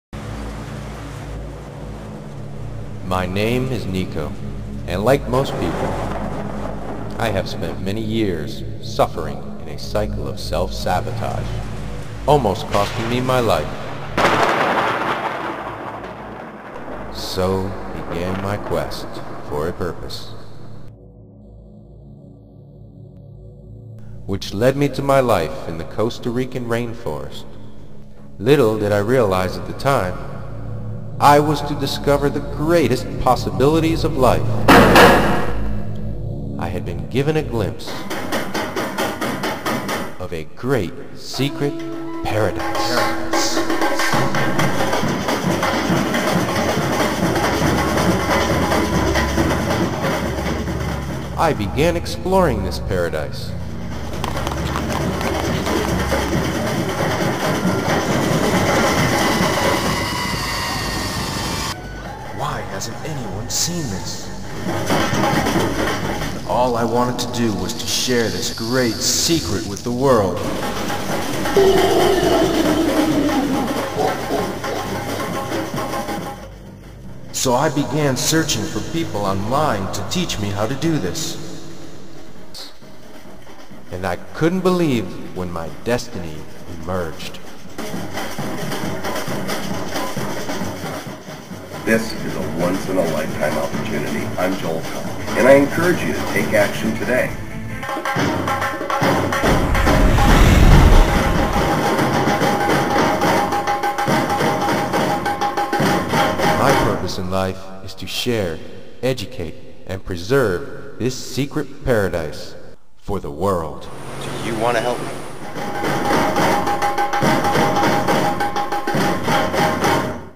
The 'Secret' Paradise- Next Internet Millionaire Audition
A Parody Of 'The Secret'. This is my audition submission for the Next Internet Millionaire reality show contest by Joel Comm. My name is Nico my girlfriend help me tirelessly for the last two weeks to shoot the footage I needed to bring this storyline to life. I hope you enjoy this look into my life in the jungle! All footage seen is my own as well as music and live samba drums. My hope is to integrate my video production skills with web 2.0 to bring the much needed awareness to this beautiful Paradise of Costa Rica. My objective is to create and develop more programs for the youth of our nations to raise awareness for the enviroment and especially the wonderful animals..what happened to the bees, anyway? Pura Vida from Costa Rica! Nico